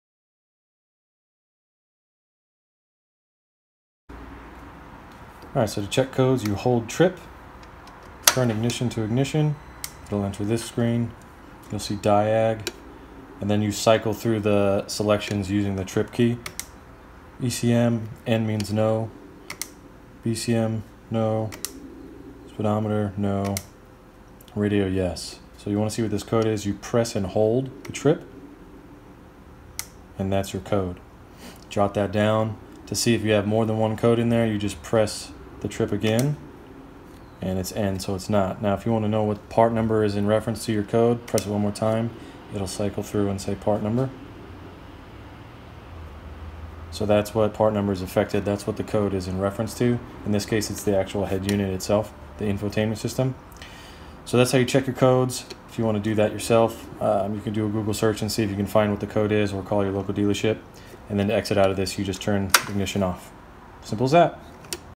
How To Check Engine Codes On Harley-Davidson’s Touring Models
This video gives a closer look at Harley-Davidson’s Touring models’ onboard diagnostic system. We’ll explain the codes you’ll see, as well as which categories to look for so you know where the fault lies, and the separate electrical systems involved on your Harley-Davidson Touring model.